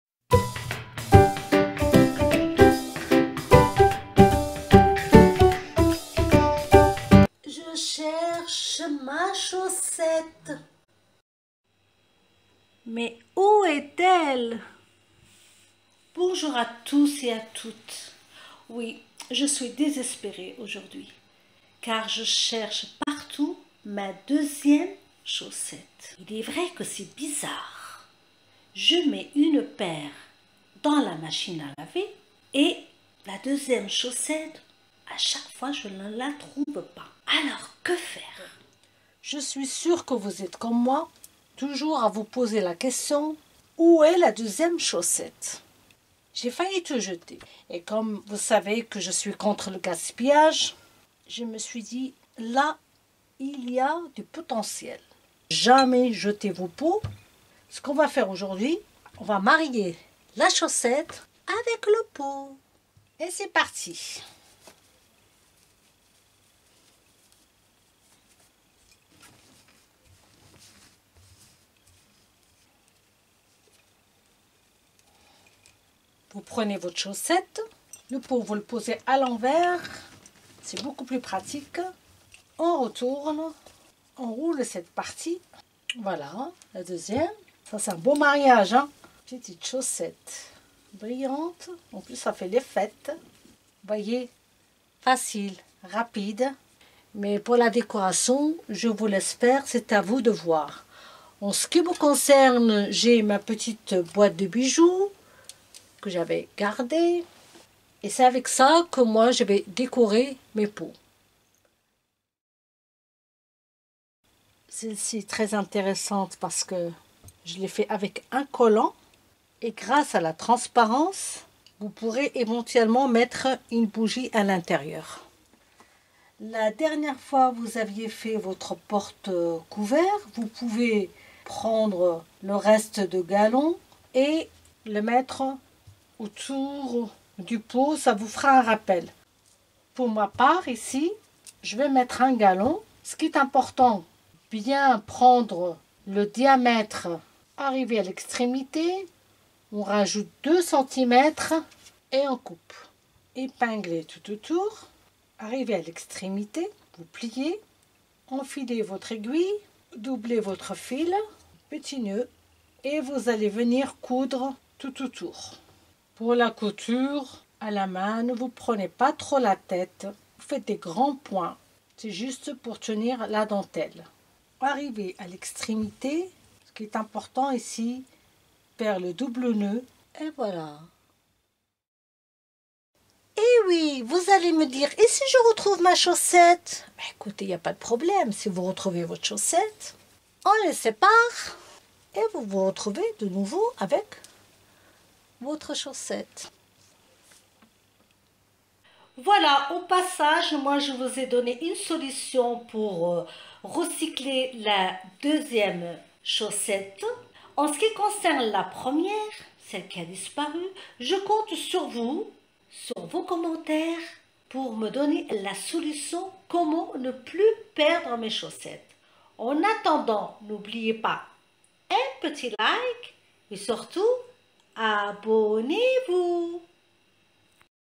DIY #11 Recyclage chaussettes. Tuto couture facile
Recyclez vos chaussettes et pots.
Une belle opportunité pour créer des pots de fêtes ou pour le rangement .
Une création rapide et facile qui nécessite juste de l'imagination au niveau des finitions .
Profitez  recyclez vos bijoux dépareillés , galons ,dentelles et boutons .
Le résultat des chaussettes collants vous surprendront . Essayer et avec toutes les couleurs .
Pour les fêtes de noël opter pour la couleur des  chaussettes  vert et rouges .
Pour réveillon or et argent . Allez lancer vous et amusez vous .

Recycle your socks and pots.
A great opportunity to create party pots or for storage .
A quick and easy creation that just requires imagination at the level of finishes.
Enjoy recycling your mismatched jewelry, stripes, lace and buttons.
The result of sticky socks will surprise you . Try and with all colors .
For Christmas holidays opt for socks in green and red color .
For gold and silver New Year’s Eve . Go throw yourself and have fun.